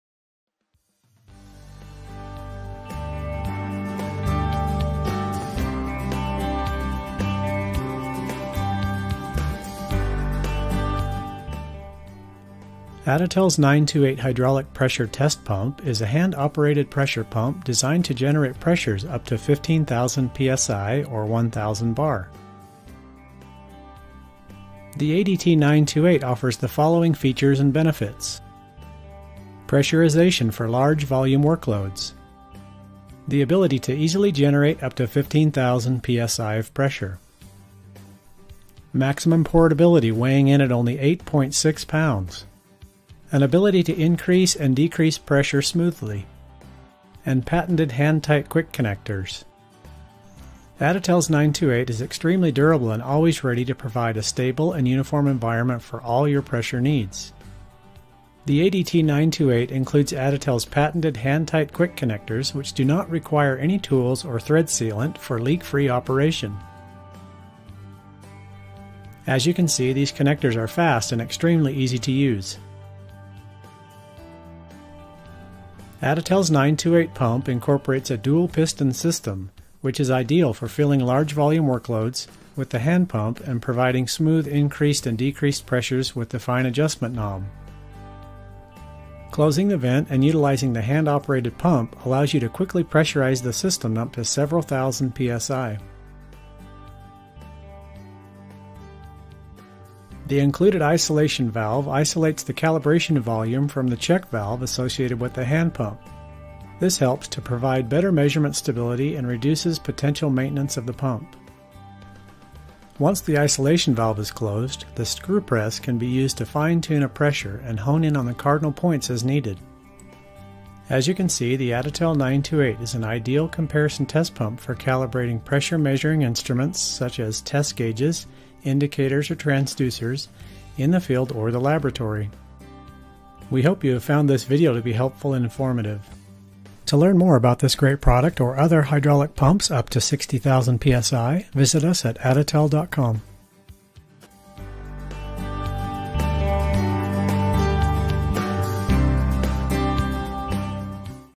Bomba de Calibración Hidráulica ADT928 1000 bar
La bomba de calibración de presión hidráulica Additel 928 es una bomba de presión manual diseñada para generar una presión de 15.000 psi (1.000 bar). Esta bomba incorpora un sistema de doble pistón que es ideal para llenar una carga de trabajo de gran volumen con la bomba manual y proporcionar un aumento y disminución suave de la presión con el  tornillo de ajuste fino.
La bomba manual Additel 928 incorpora una válvula de aislamiento que aísla el volumen de calibración de la válvula de retención asociada con la bomba manual. Debido a que la válvula de retención a menudo puede ser una fuente de fugas y mantenimiento, la válvula de aislamiento proporciona mediciones más estables y reduce el mantenimiento potencial de la bomba. Dos conectores rápidos para ser apretados a mano instalados en la bomba permiten una fácil conexión y desconexión a los equipos bajo prueba sin la necesidad de cinta de PTFE o llaves.
La bomba de calibración Additel 928 es una bomba de prueba de comparación ideal para calibrar instrumentos de medición de presión como manómetros, indicadores o transductores tanto en campo como en el laboratorio.